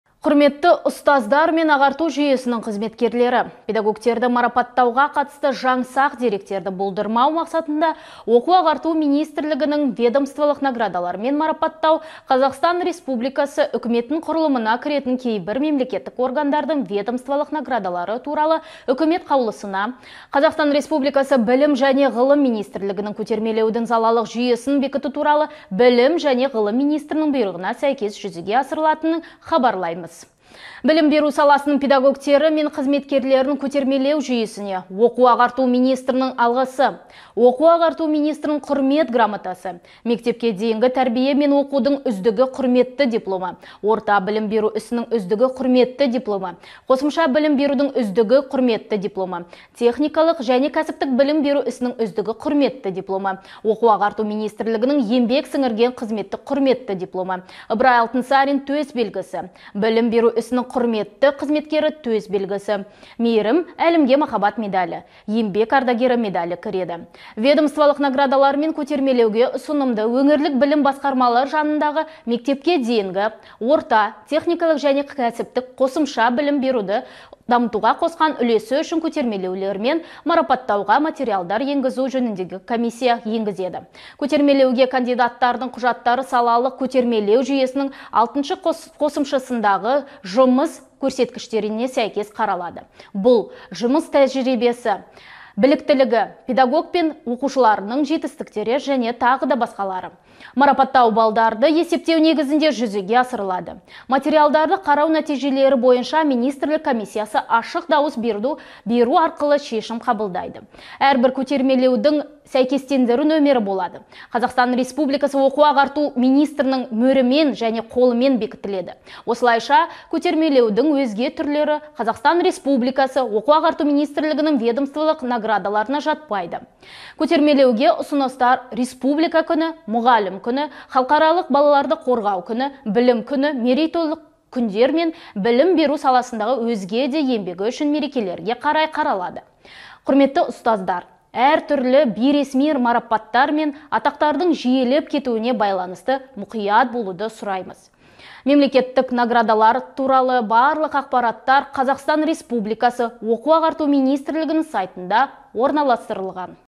Білім беру саласының педагогтері мен қызметкерлерін көтермелеу жүйесі туралы
ҚР Оқу-ағарту министрлігінің баспасөз хатшысы білім беру саласының педагогтері мен қызметкерлерін көтермелеу жүйесі туралы